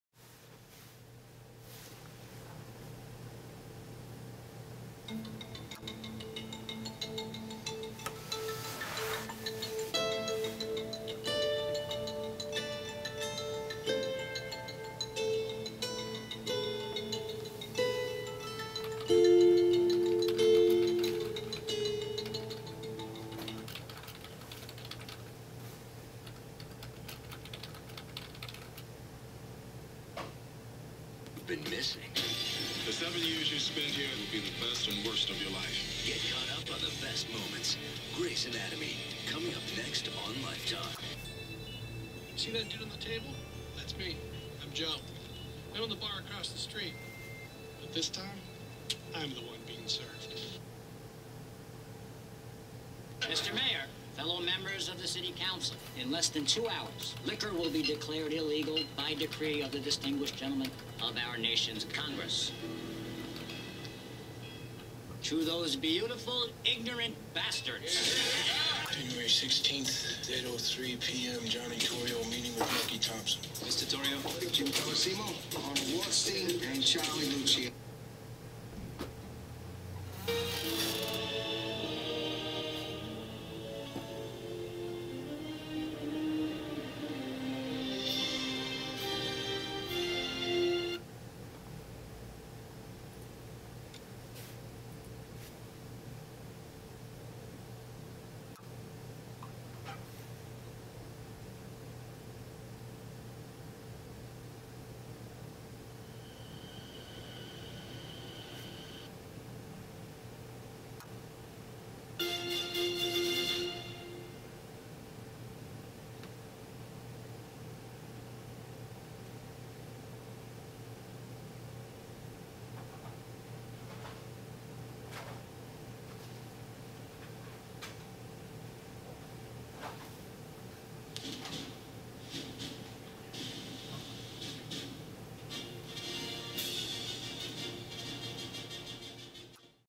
Niveus Crystal Touch Interface for DirecTV
Niveus created a touch interface to bring DirecTV HD Recordings on to an All-In-One Touch PC. The application leverages Niveus Link technology and securely streams any recorded show from the DirecTV HD DVR.
See more demos and learn more about the Niveus Connected Platform